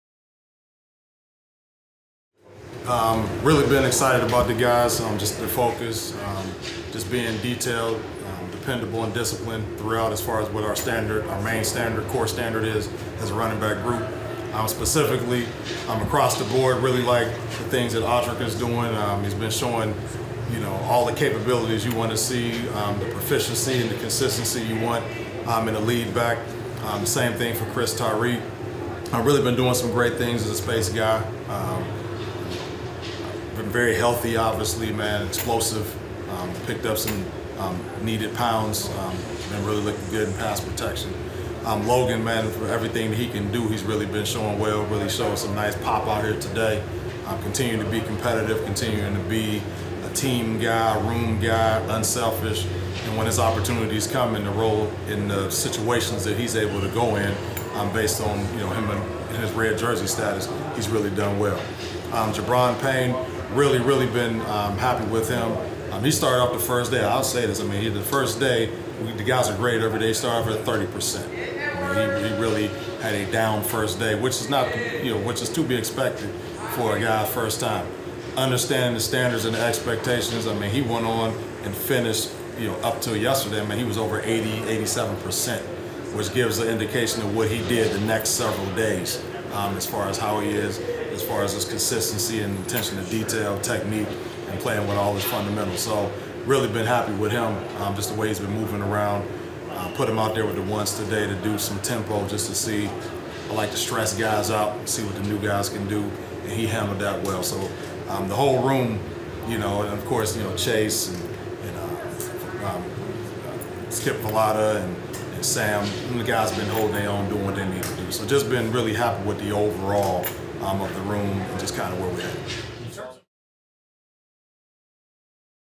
Deland McCullough Breaks Down Notre Dame Backs In Camp
Notre Dame running backs coach Deland McCullough breaks down how his backs have performed thus far in fall camp.

Stay locked into Irish Breakdown for all the latest news and analysis about Notre Dame